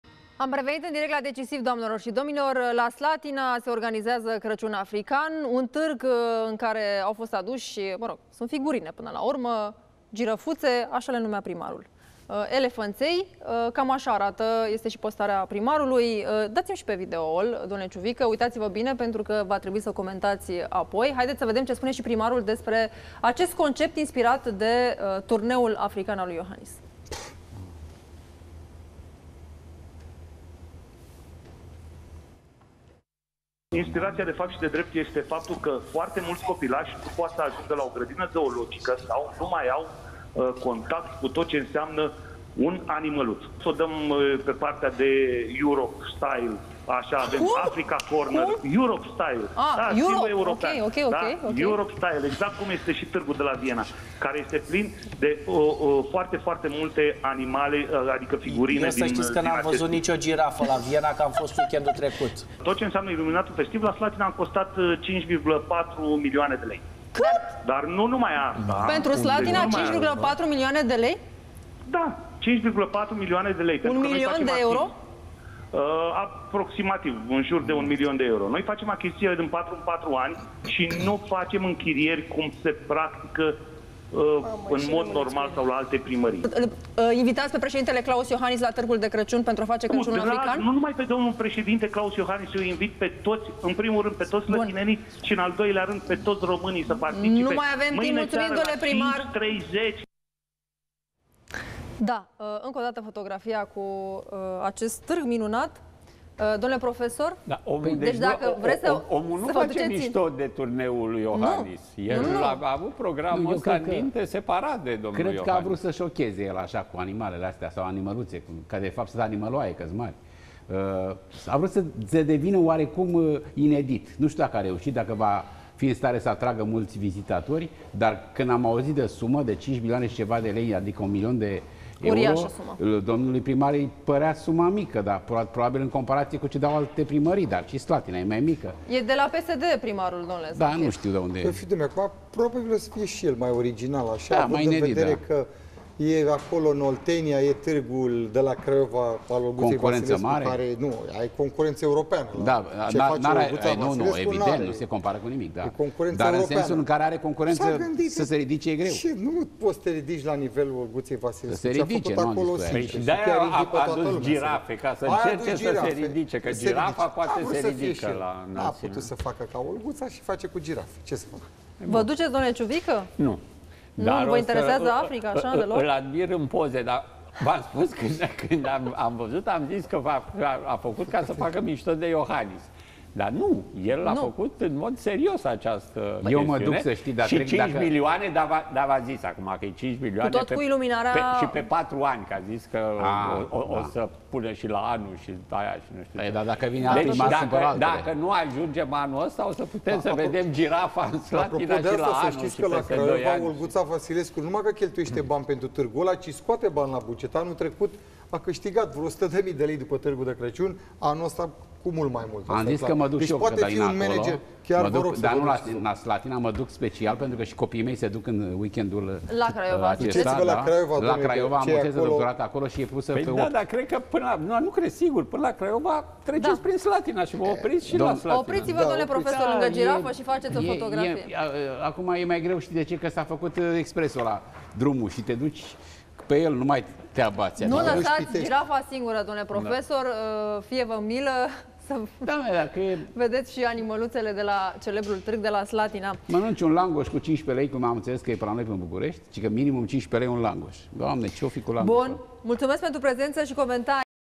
Orașul din România în care se organizează un Târg de Crăciun african. Un milion de euro a costat
Orașul din România în care se organizează un Târg de Crăciun ”african” Un milion de euro a costat investiția